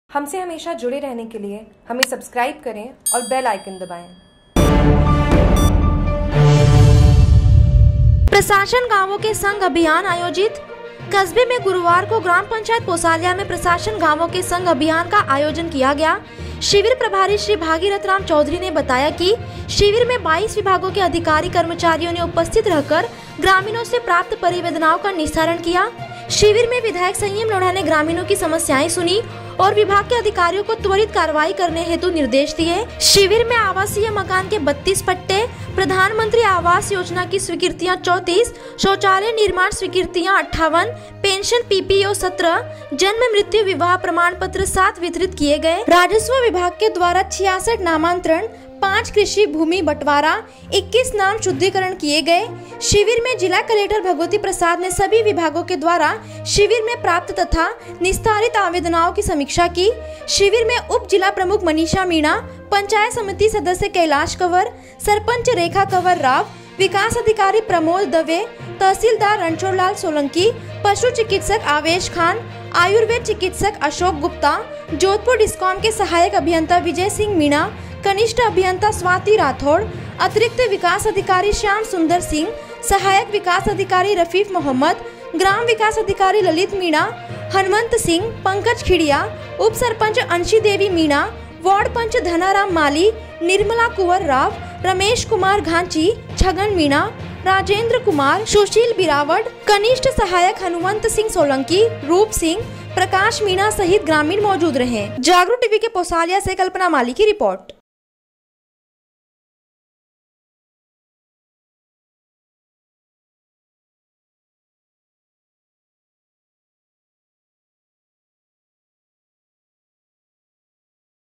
पोसालिया में प्रशासन गांव अभियान के तहत लगा शिविर
कस्बे में गुरुवार को ग्राम पंचायत पोसालिया में प्रशासन गांवों के संग अभियान का आयोजन किया गया l शिविर प्रभारी श्री भागीरथराम चौधरी ने बताया कि शिविर में 22 विभागों के अधिकारी, कर्मचारियों ने उपस्थित रहकर ग्रामीणों से प्राप्त परिवेदनाओं का निस्तारण किया l शिविर में विधायक संयम लोढ़ा ने ग्रामीणों की समस्याएं सुनी और विभाग के अधिकारियों को त्वरित कार्रवाई हेतु निर्देश दिए lशिविर में आवासीय मकान के 32 पट्टे, प्रधानमंत्री आवास योजना की स्वीकृतिया 34 ,शौचालय निर्माण स्वीकृतिया 58 ,पेंशन पीपीओ 17, जन्म मृत्यू विवाह प्रमाण पत्र 7 वितरित किए गए l राजस्व विभाग के द्वारा 66 नामांतरण ,पांच कृषि भूमि बटवारा, 21 नाम शुद्धीकरण किए गए l शिविर में जिला कलेक्टर भगवती प्रसाद ने सभी विभागो के द्वारा शिविर में प्राप्त तथा निस्तारित आवेदनो की समीक्षा की l शिविर में उप जिला प्रमुख मनीषा मीणा ,पंचायत समिति सदस्य कैलाश कंवर ,सरपंच रेखा कंवर राव ,विकास अधिकारी प्रमोद दवे, तहसीलदार रणछोड़ लाल सोलंकी ,पशु चिकित्सक आवेश खान ,आयुर्वेद चिकित्सक अशोक गुप्ता ,जोधपुर डिस्कॉम के सहायक अभियंता विजय सिंह मीणा, कनिष्ठ अभियंता स्वाति राठौड़ ,अतिरिक्त विकास अधिकारी श्याम सुंदर सिंह,सहायक विकास अधिकारी रफीक मोहम्मद ,ग्राम विकास अधिकारी ललित मीणा ,हनवन्त सिंह,पंकज खिडिया, उप सरपंच अंशी देवी मीणा ,वार्ड पंच धनाराम माली निर्मला कुंवर राव,रमेश कुमार घांची, छगन मीणा ,राजेंद्र कुमार ,सुशील बिरावट ,कनिष्ठ सहायक हनुवंत सिंह सोलंकी ,रूप सिंह, प्रकाश मीणा सहित ग्रामीण मौजूद रहे